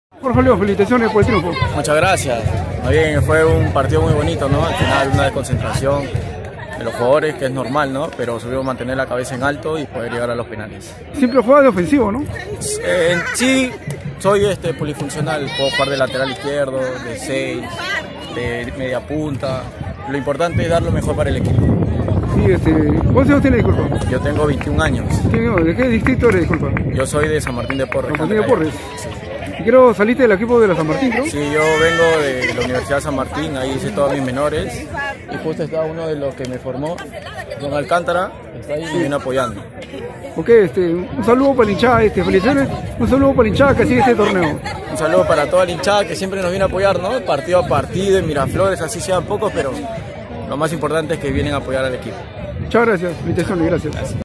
declara Jorge Leon
declara Jorge león, jugador del DIM de Miraflores, luego de clasificar a la etapa nacional de la copa Perú, el 17/8/19 en estadio Segundo Aranda Torres, Huacho.